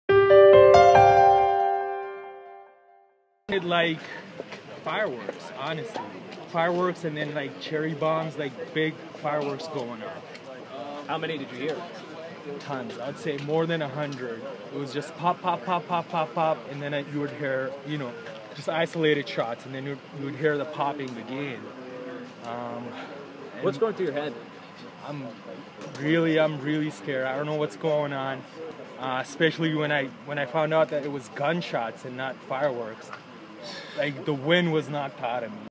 'It sounded like fireworks'
(Boston Globe) Eyewitness in Watertown describes the sounds and scene. By Eric Moskowitz/Globe Staff